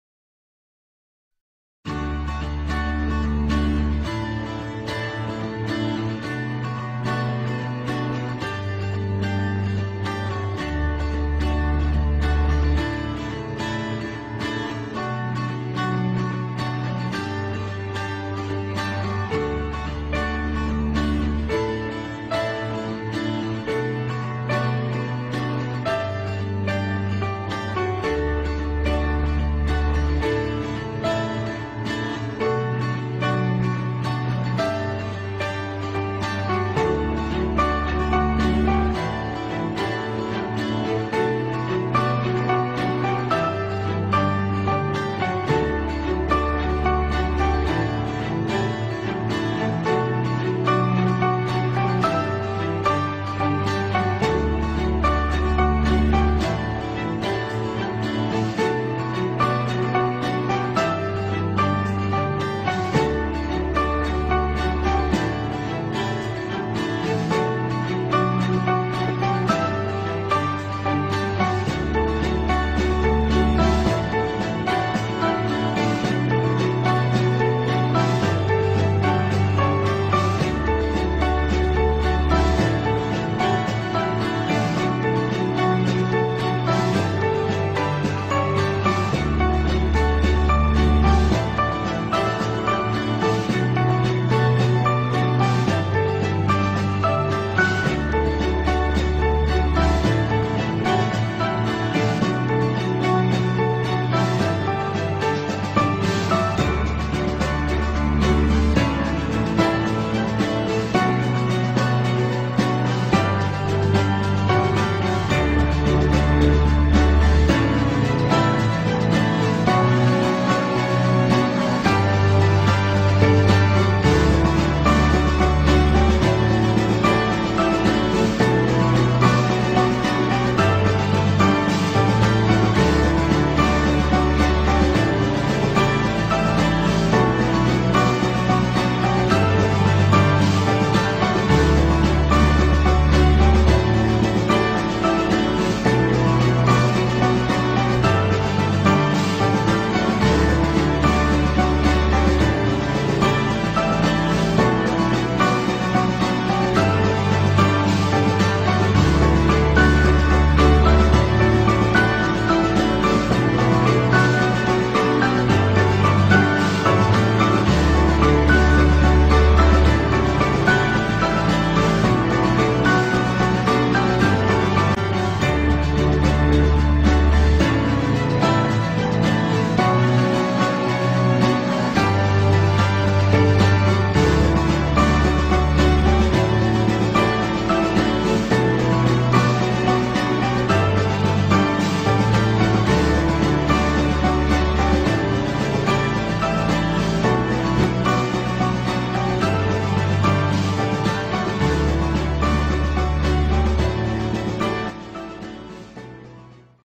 Department of Computer Science and Engineering || BSMRSTU Documentary || BSMRSTU CSE 2K18
Bangabandu Sheikh Mujibur Rahman Science and Technology University is a public university located in Gobra, Gopalganj, Bangladesh. It was established in 2001 and named after Bangabandhu Sheikh Mujibur Rahman. This video is contain the Nobin Boron program of 2k18.